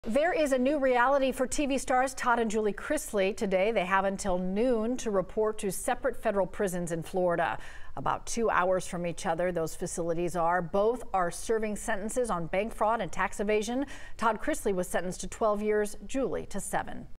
Chrisley's to report to prison today
They have until noon today to report to separate federal prisons in Florida.